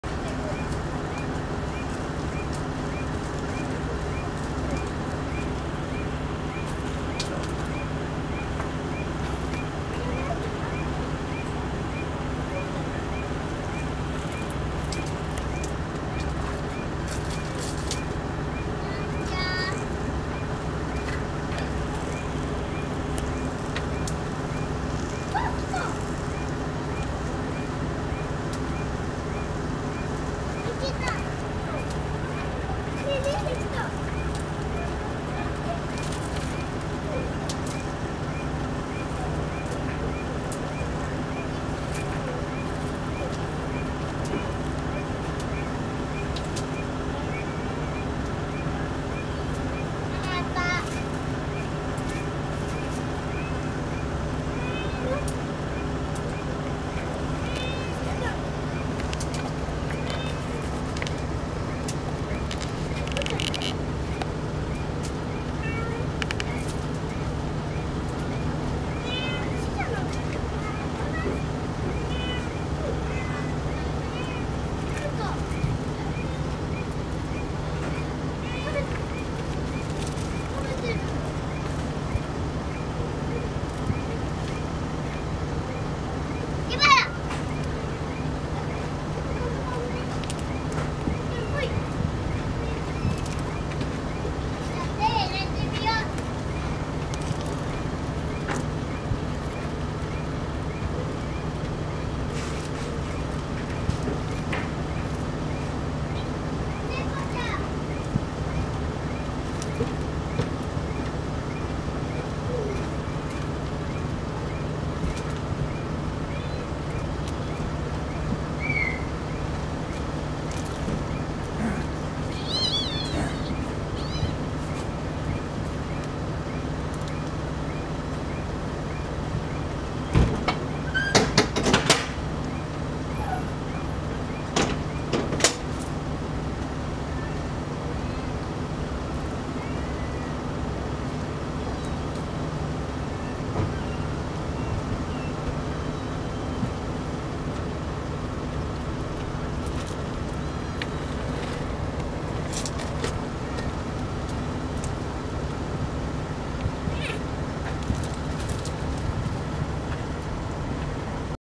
かわいい子猫と子猫のとっくみあい Kittens play together
子猫と子猫がじゃれ合ってとっくみあいをする姿を動画に収めました。
子猫ってなんでこんなにかわいいのでしょうね。
愛されるために生まれてきた感じ。
ただただ癒しｗ

usualy japan は日本各地の日常風景や観光、文化、風土などを動画で紹介しています。